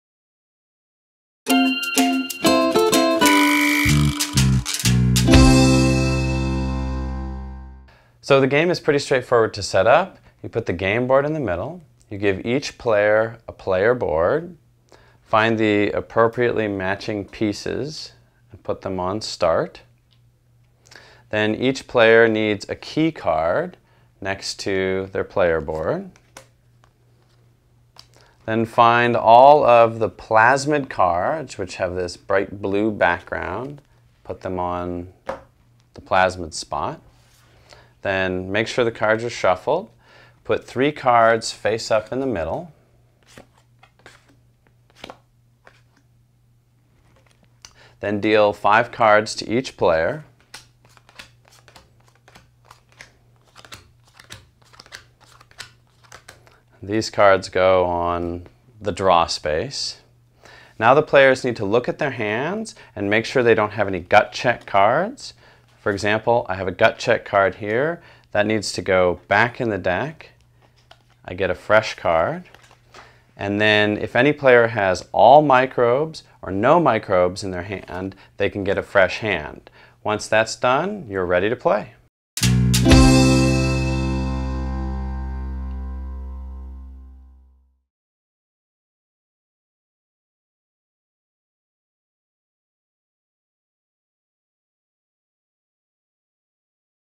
GUT CHECK: How to Set Up
Learn to set-up GUT CHECK: The Microbiome Game -
A game where you build up your microbiome and sabotage your friends with the plague, botulism, and more!